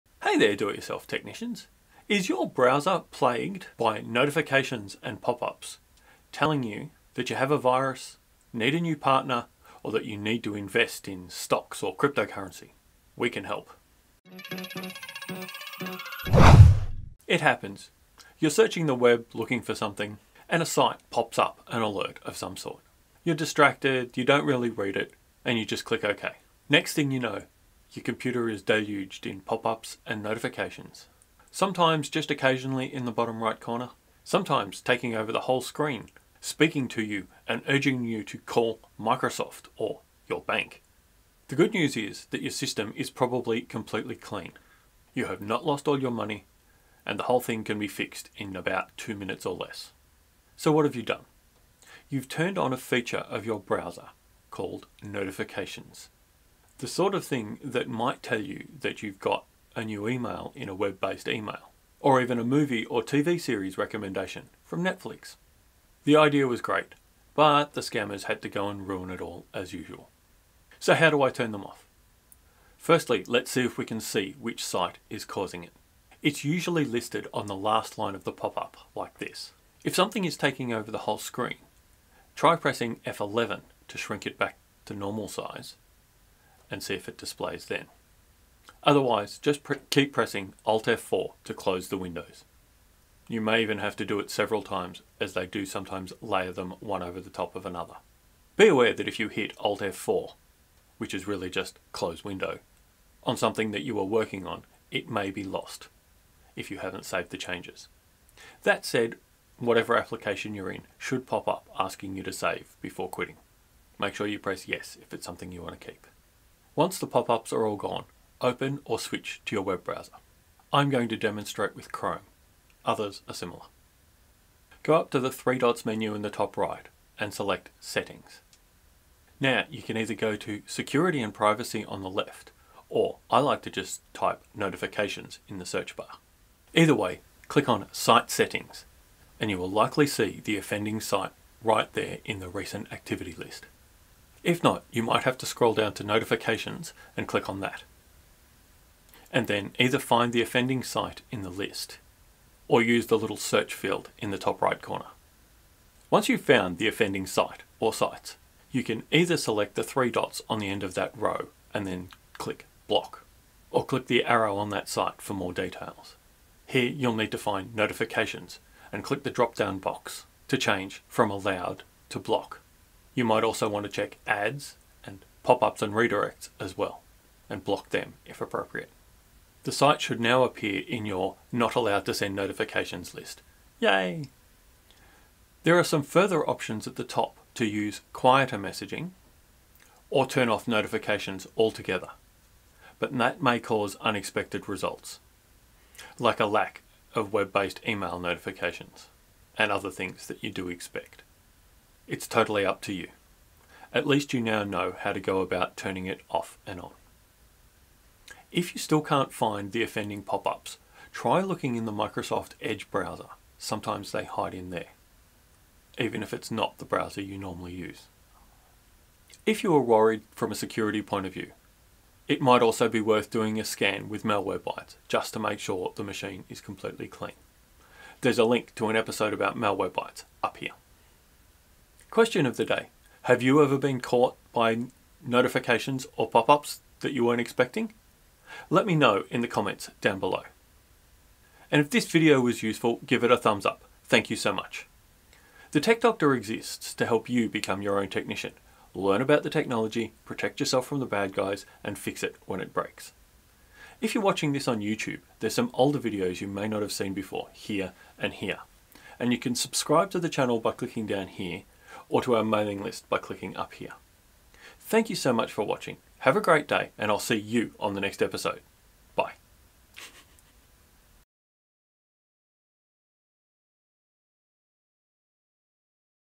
199: Virus? Scam? No just Browser Notifications, and they are easy to get rid of.
In this episode, we look at Browser Notifications, a great feature of modern web browsers exploited and ruined by the spammers and scammers.

Many of those pop up windows are nothing more than a browser notification and they are really easy to turn off. Go to your browser Settings, find notifications, find the offending site (it's listed in pop-up) and block it from being able to have notifications.

Also, be aware what you allow to send notifications. More than likely it's only a handful of sites, if any.

Fix it when it breaks